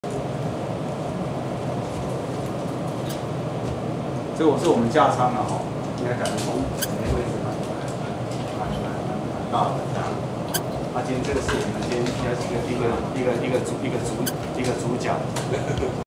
2015.8.3 基隆港 東四碼頭 海巡署CG128宜蘭艦 104年北部地區擴大威力掃蕩
2015.8.3 基隆港 東四碼頭 行政院海岸巡防署 CG128宜蘭艦 海巡署3000噸級巡防救難艦
行政院海岸巡防署 海洋巡防總局 104年北部地區擴大威力掃蕩